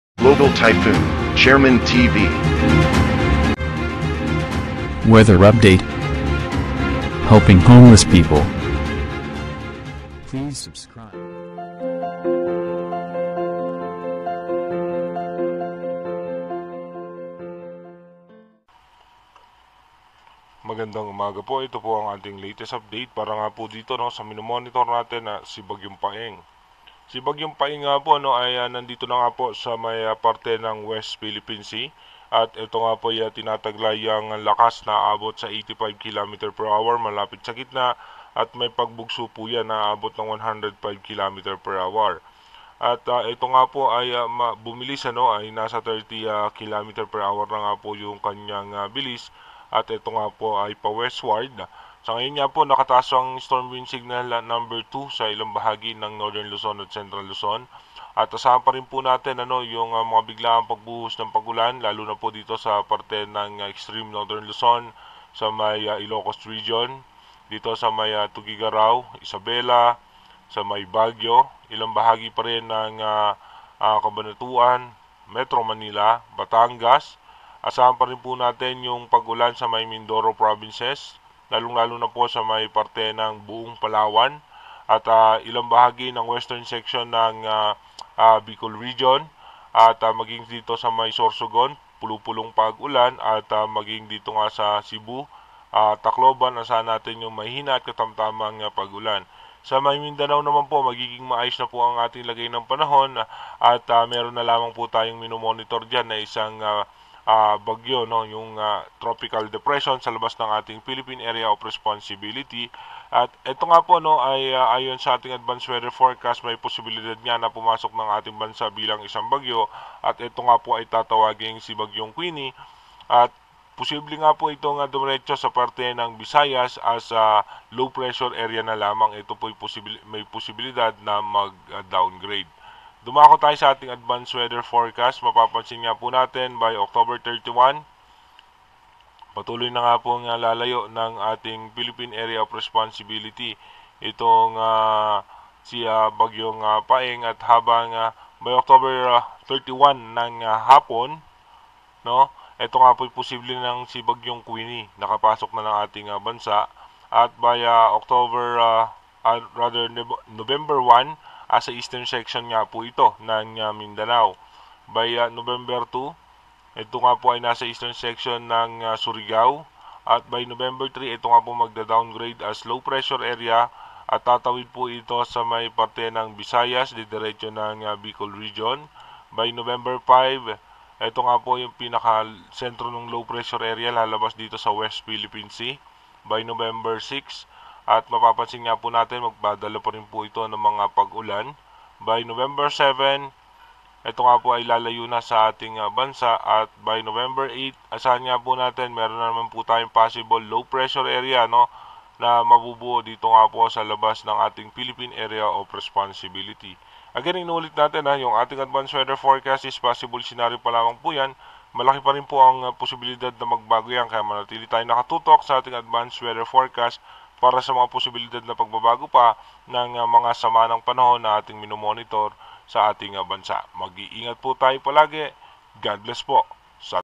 BAGYONG "PAENG" ASA WEST PH SEA NA PRE-QUEENIE POSSIBLENG PUMASOK BUKAS SA PAR report#58
LATEST UPDATE OCTOBER   30,2022 AS OF  07:00 AM

***SYNOPSIS: The center of Tropical Storm “PAENG” was estimated based on all available data at 85 km West Northwest of Iba, Zambales (15.7 °N, 119.3 °E )Maximum sustained winds of 85 km/h near the center and gustiness of up to 105 km/h Moving West Northwestward at 30 km/h

SIGNAL#2
Luzon

SIGNAL #1
Luzon

 The whole archipelago Light to Moderate East to Southeast Slight to Moderate / (0.6 to 2.1 meters)

DOST-PAGASA SITE
WINDY APPS
ZOOM EARTH